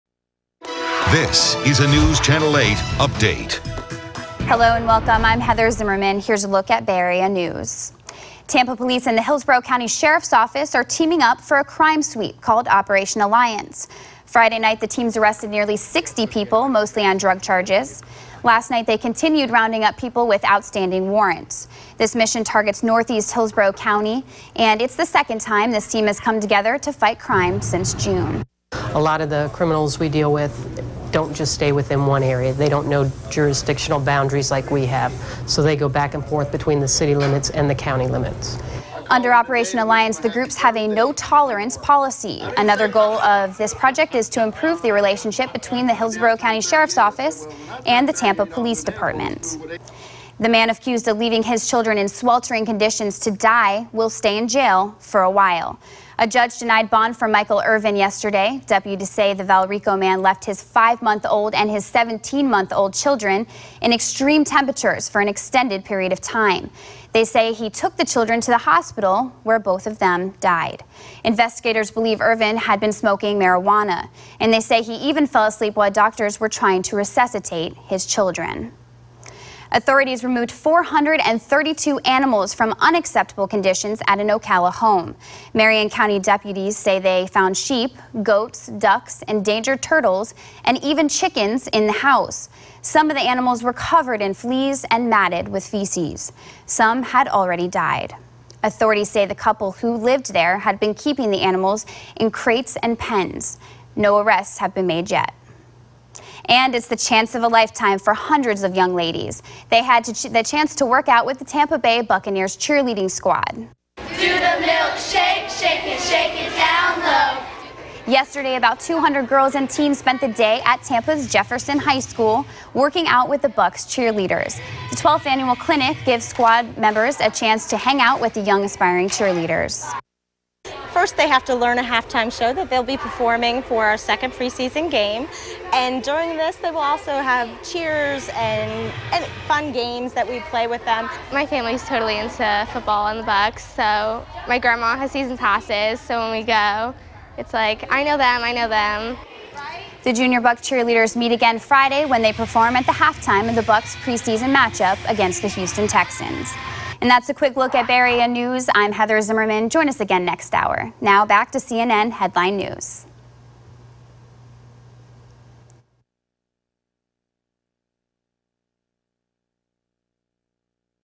A CNN Local News update from News Channel 8
This is a anchor demo reel from News channel 8.